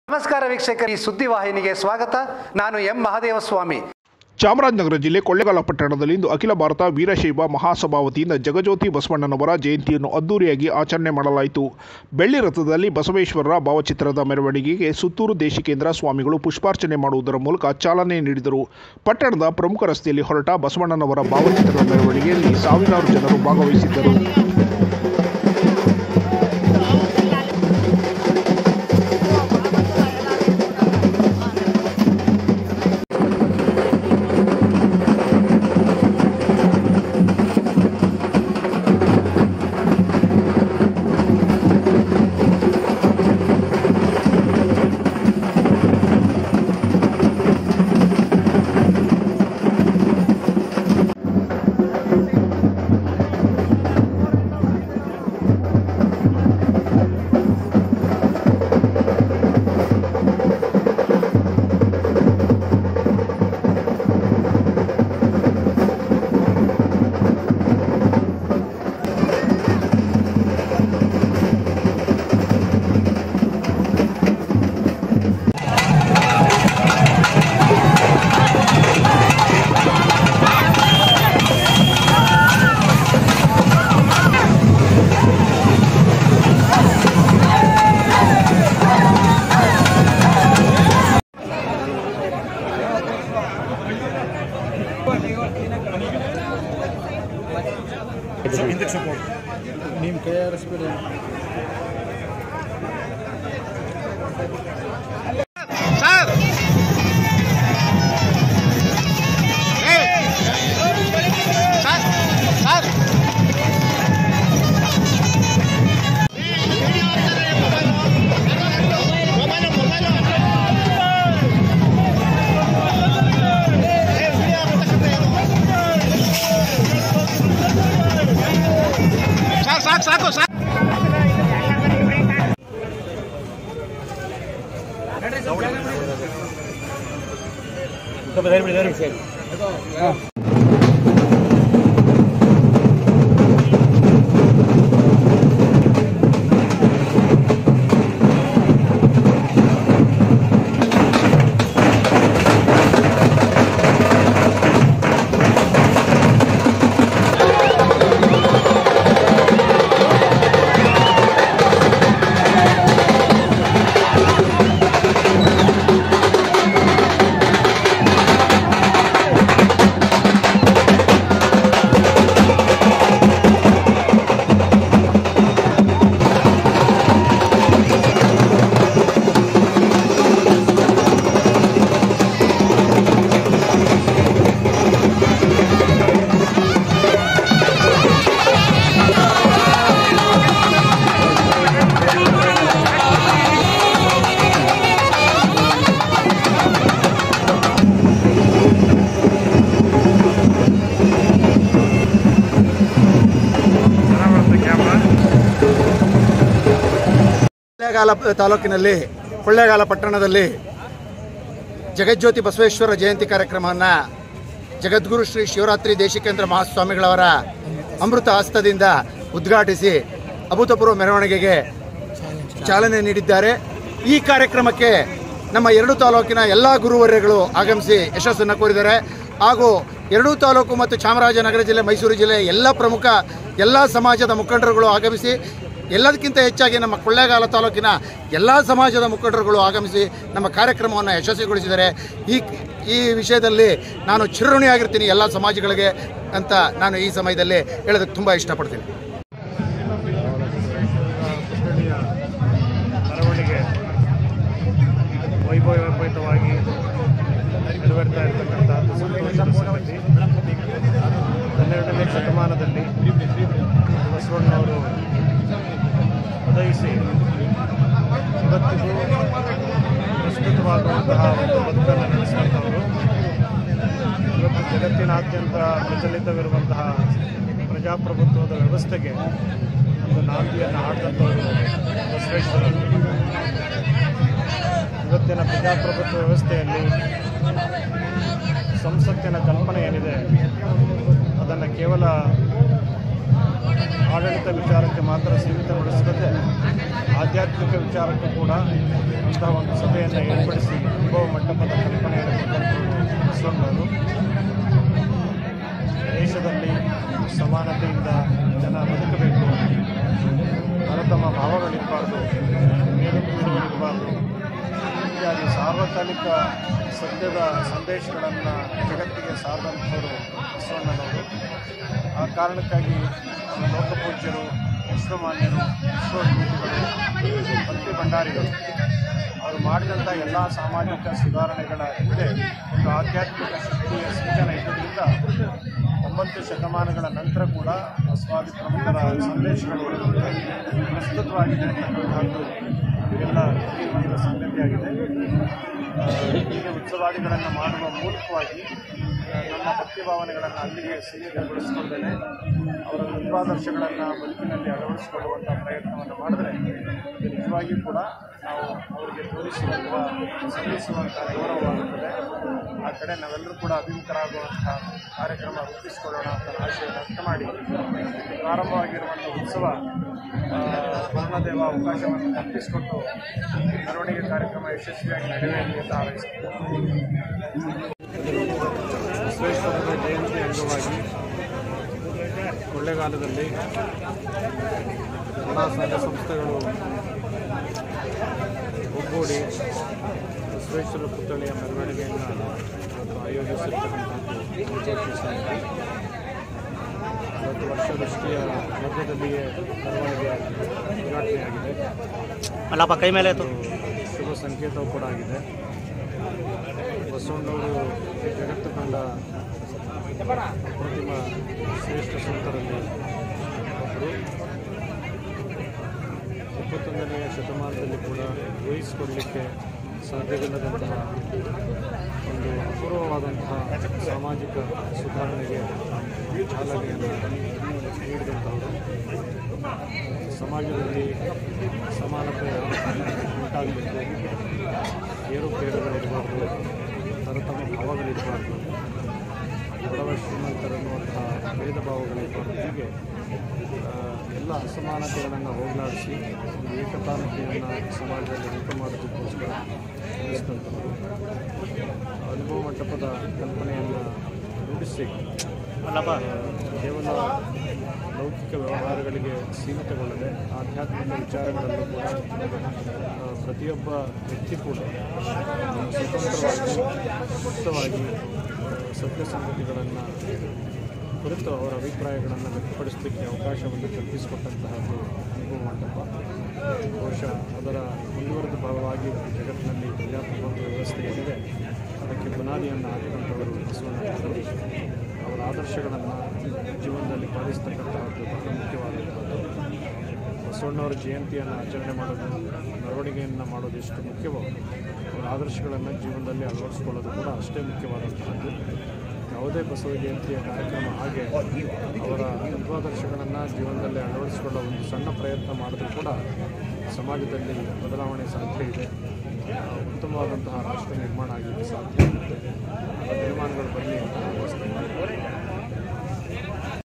ಚಾಮರಾಜನಗರ ಜಿಲ್ಲೆ ಕೊಳ್ಳೇಗಾಲ ಪಟ್ಟಣದಲ್ಲಿ ಅಖಿಲ ಭಾರತ ವೀರಶೈವ ಮಹಾಸಭಾ ವತಿಯಿಂದ ಇಂದು ಜಗಜ್ಯೋತಿ ಬಸವಣ್ಣನವರ
ಚಾಮರಾಜನಗರ ಜಿಲ್ಲೆ ಕೊಳ್ಳೇಗಾಲ ಪಟ್ಟಣದಲ್ಲಿ ಅಖಿಲ ಭಾರತ ವೀರಶೈವ ಮಹಾಸಭಾ ವತಿಯಿಂದ ಇಂದು ಜಗಜ್ಯೋತಿ ಬಸವಣ್ಣನವರ ಜಯಂತಿಯನ್ನು ಅದ್ದೂರಿಯಾಗಿ ಆಚರಣೆ ಮಾಡಲಾಯಿತು. ಬೆಳ್ಳಿ ರಥದಲ್ಲಿ ಬಸವೇಶ್ವರರ ಭಾವಚಿತ್ರಕ್ಕೆ ಸುತ್ತೂರು ದೇಶೀಕೇಂದ್ರ ಸ್ವಾಮಿಗಳು ಪುಷ್ಪಾರ್ಚನೆ ಮಾಡುವುದರ ಮೂಲಕ ಮೆರವಣಿಗೆಗೆ ಚಾಲನೆ ದೊರಕಿಸಿ ಕೊಟ್ಟರು. ಪಟ್ಟಣದ ಪ್ರಮುಖ ರಸ್ತೆಗಳಲ್ಲಿ ಹೊರಟ ಭಾವಚಿತ್ರ ಮೆರವಣಿಗೆಯಲ್ಲಿ ಸಾವಿರಾರು ಜನರು ಭಾಗವಹಿಸಿದರು. ವಿವಿಧ ಕಲಾ ತಂಡಗಳಿಂದ ಡೊಳ್ಳುಕುಣಿತ, ಜಾನಪದ ಕಲಾವಿದರ ನೃತ್ಯ ನೋಡುಗರ ಕಣ್ಮನ ಸೆಳೆಯಿತು.